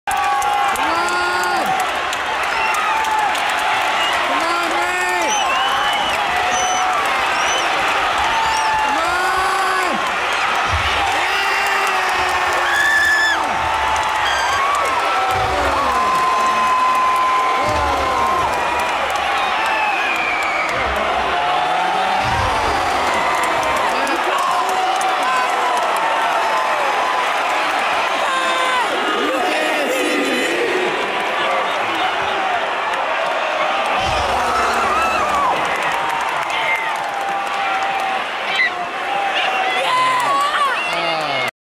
WWE Friday Night Smackdown Cena & Mysterios Vs The Usos & Roman Reigns Dark Match
After Smackdown Went off the Air in Tampa Fl